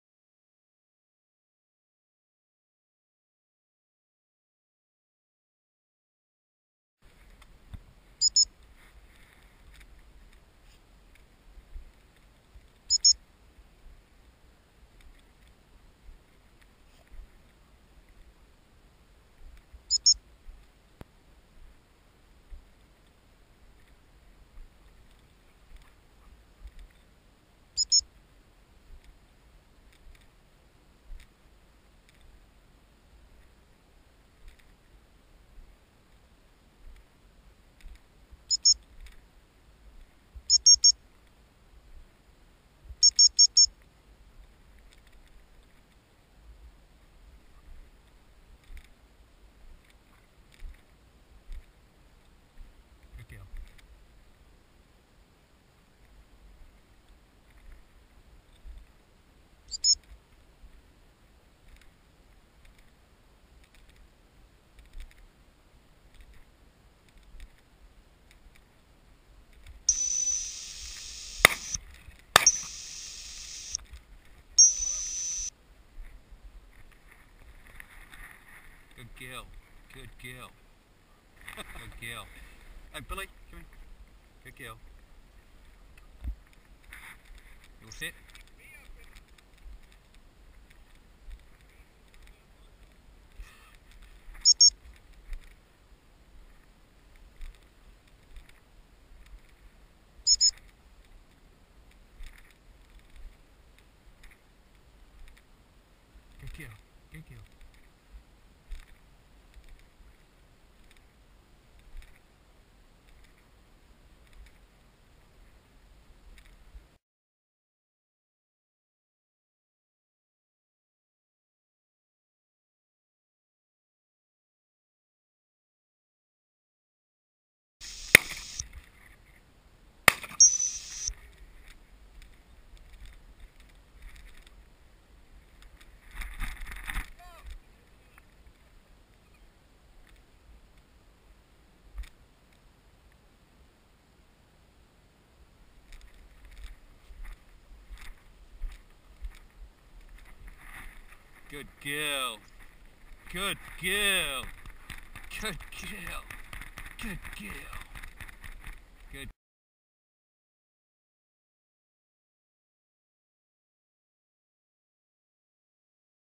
Hunting Rabbits Dog and Gun
Billie hunting and flushing rabbits for the Gun to shoot and Billie retrieving the shot rabbit.